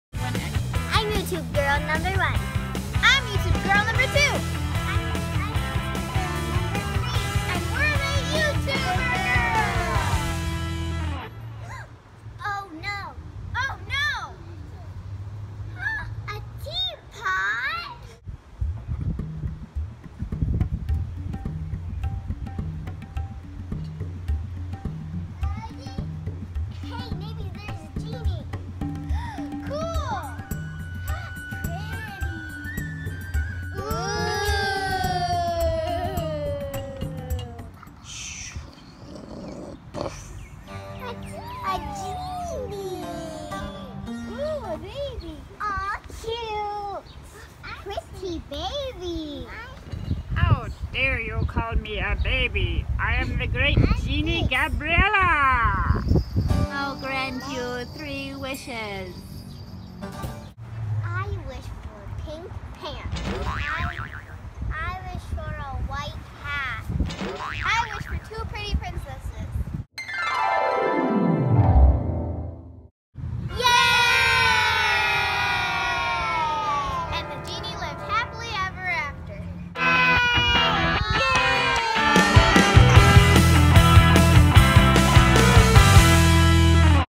Genie Girls
Genie Gabriella grants 3 wishes.